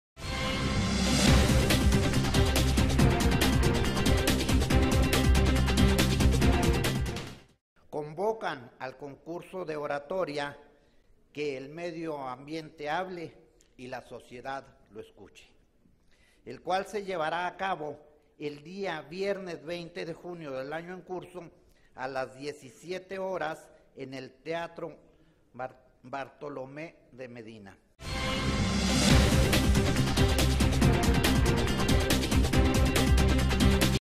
SesionOrdinaria Presenta Antonio Chávez tema "Que medio ambiente hable y que sociedad escuche"
SesionOrdinaria Presenta Antonio Chávez Barraza tema "Que el medio ambiente hable y que la sociedad lo escuche"